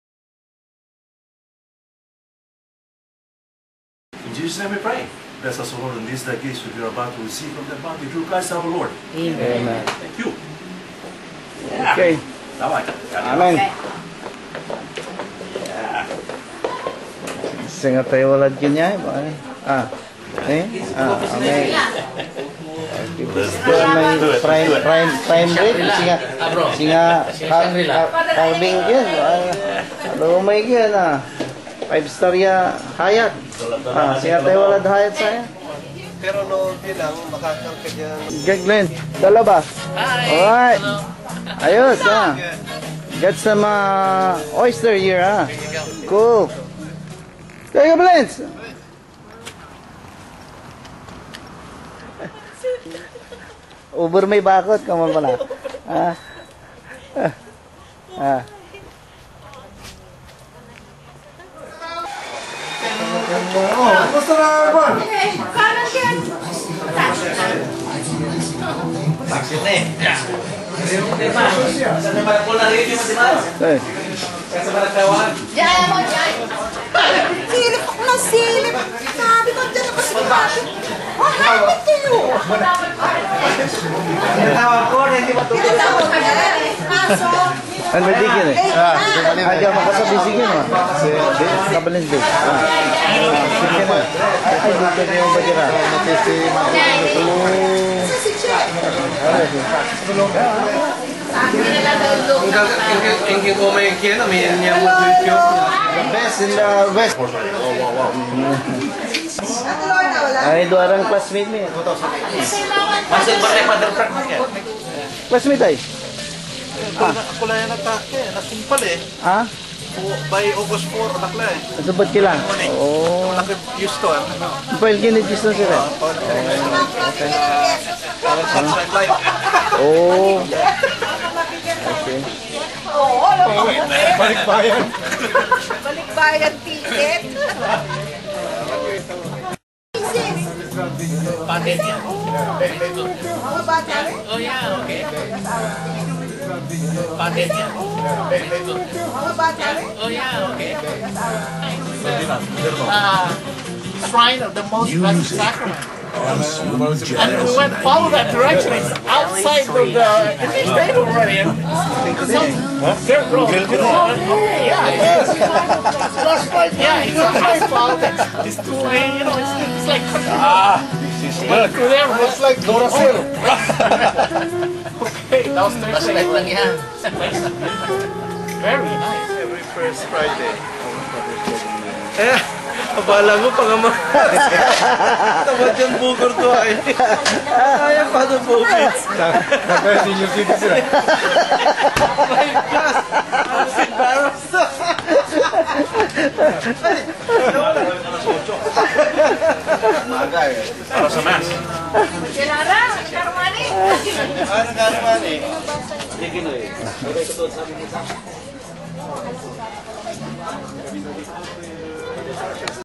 K.I.R. 2007
...1ST DAY ...WELCOME PARTY @ KABALENS BENJIE's HOME SWEET HOME...
CHULA VISTA,CALIFORNIA

3RD KABALENS INTERNATIONAL REUNION
SAN DIEGO,CALIFORNIA